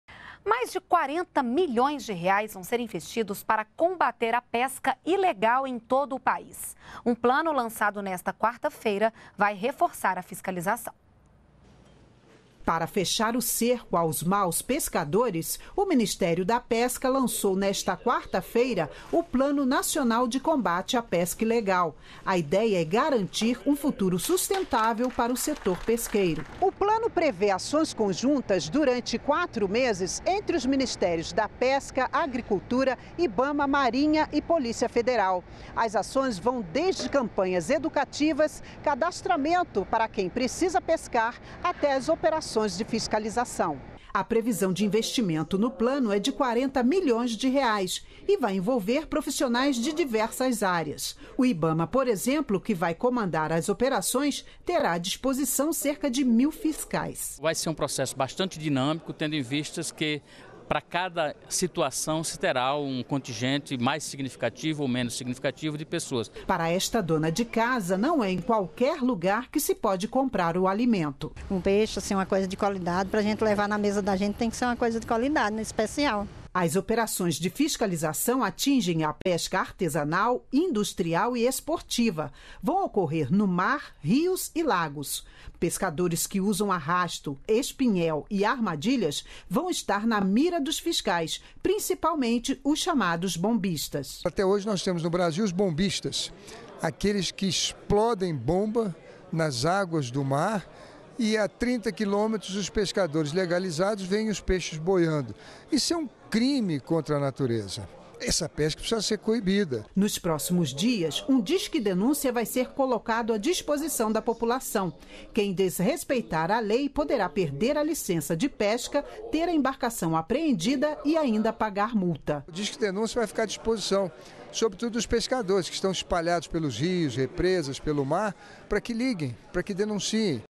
Operações de fiscalização da pesca ilegal recebem investimentos de mais de R$ 40 milhões
NBR NOTÍCIAS - 04.12.13: O Plano Nacional de Combate à Pesca Ilegal, lançado nesta quarta-feira (4), vai combater irregularidades em diversos tipos de pesca. O objetivo do plano é garantir um futuro sustentável para o setor pesqueiro brasileiro. A operação, com duração de quatro meses, terá o apoio do Ibama, Marinha, Ministério da Pesca e Aquicultura, Ministério da Agricultura, Pecuária e Abastecimento e da Polícia Federal. Nas ações, serão realizadas campanhas de esclarecimentos, cadastramento e fiscalização. Quem desrespeitar a lei pode receber multa, perder a licença de pesca e ter a embarcação apreendida.